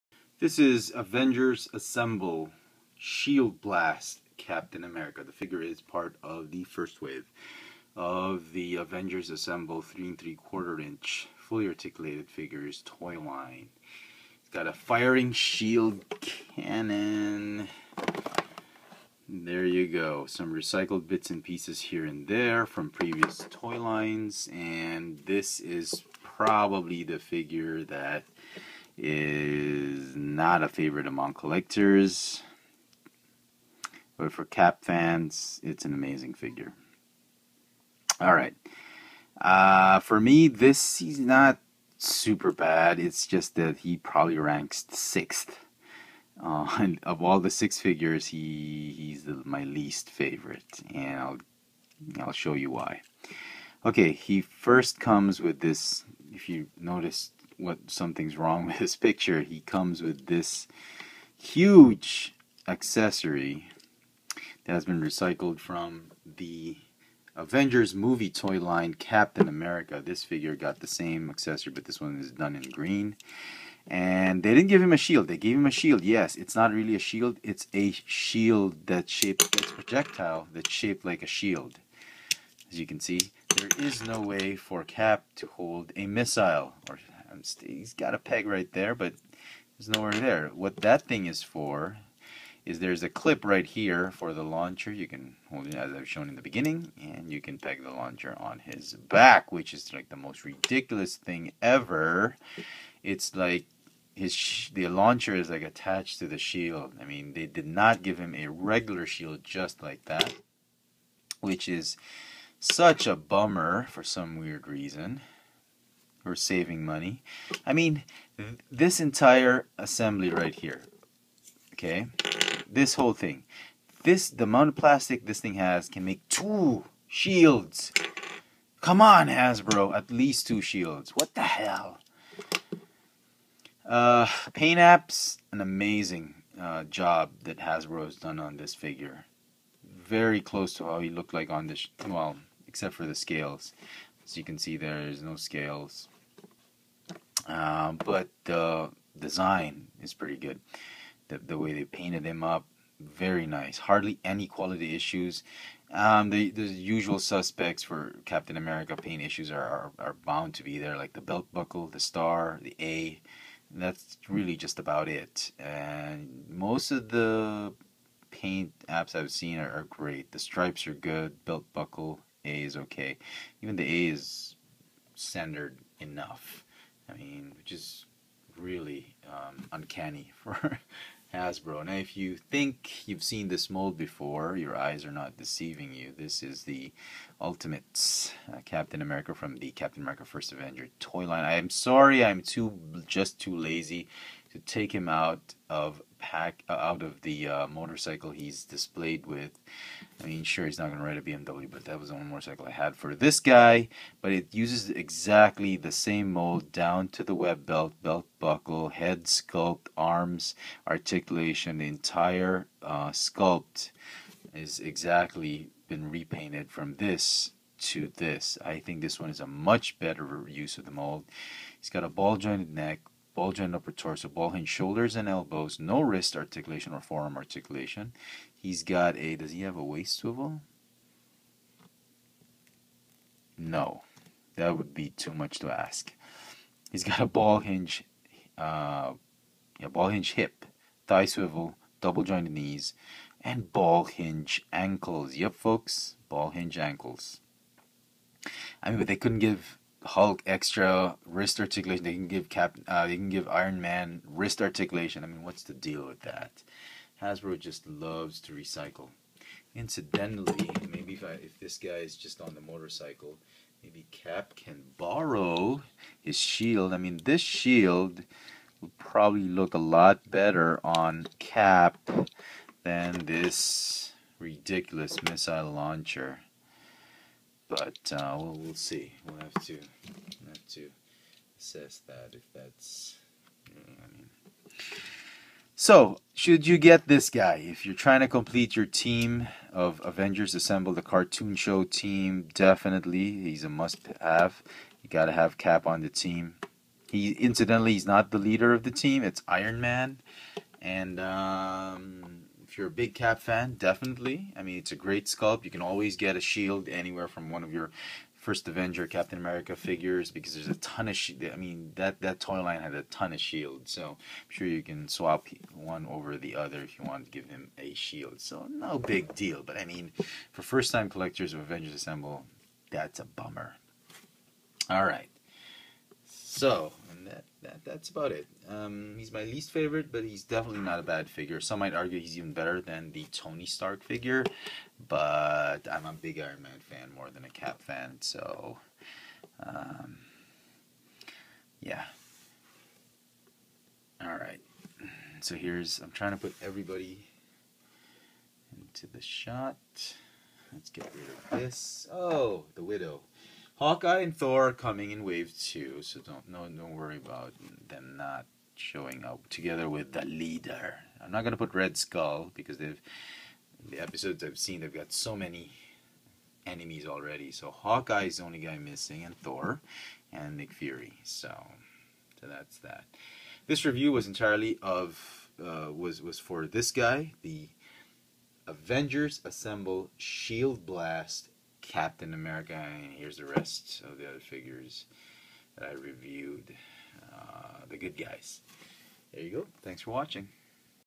Avengers Assemble Captain America Shield Blast
any cap figure needs a decent shield, right ?  wtf Hasbro?!